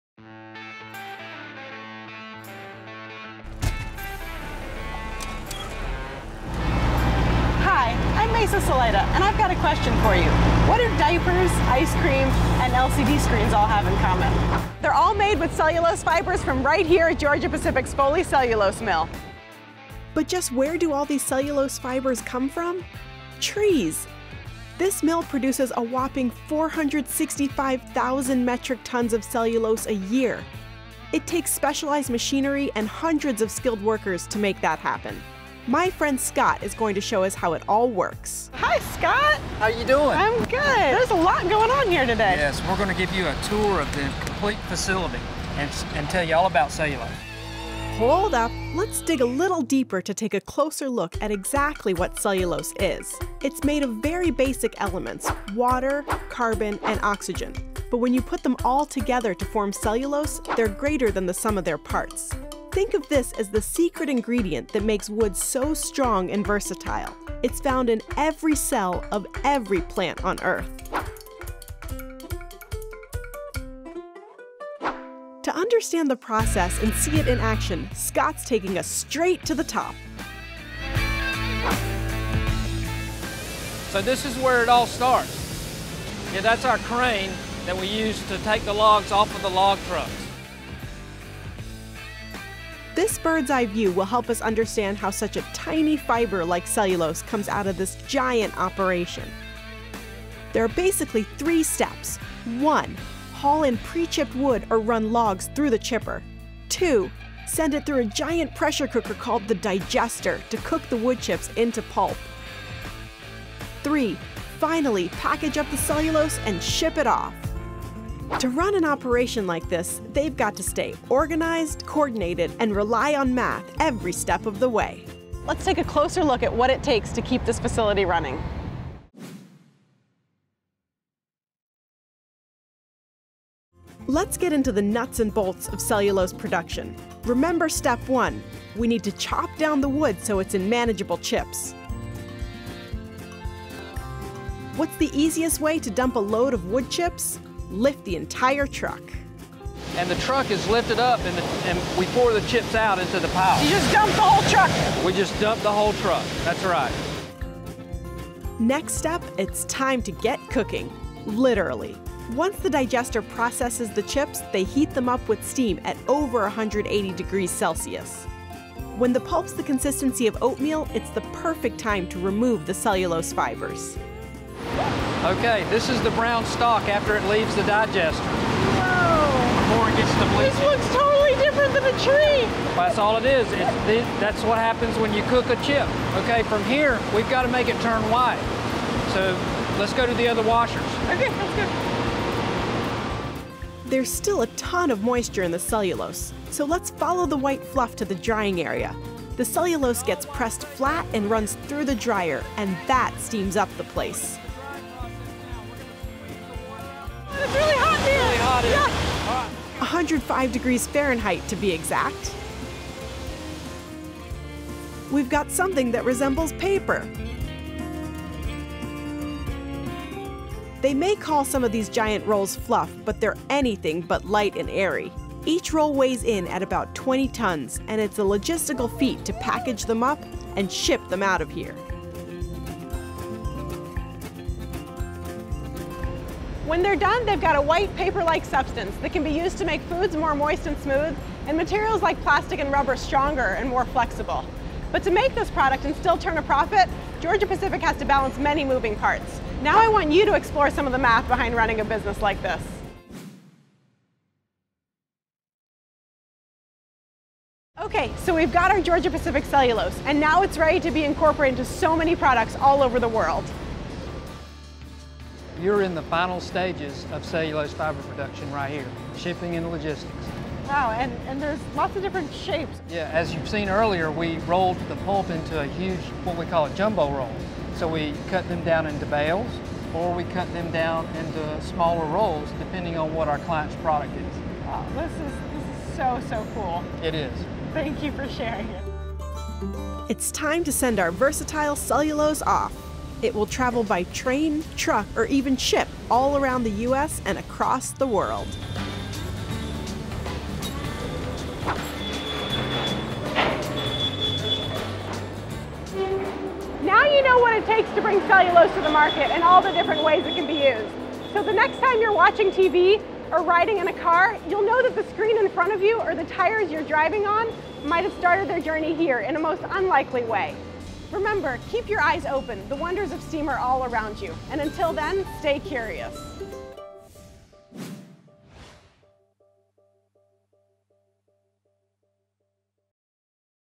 Math | Soup to Nuts at Georgia-Pacific (Activity Breaks)
What do diapers, ice cream and LCD screens have in common?  They’re all made with cellulose fibers from right here at Georgia Pacific’s Foley Cellulose Mill.

Just where do all of these cellulose fibers come from?

We’ll explore how this mill extracts a whopping 465 thousand metric tons of cellulose a year from trees!

Public Affairs Manager Scott Mixon shows us all the specialized machinery and hundreds of skilled workers needed to make that happen.

During this episode, hosted by Dr. Meisa Salaita, we follow Scott as he shows us the whole process… from soup to nuts.

gp.com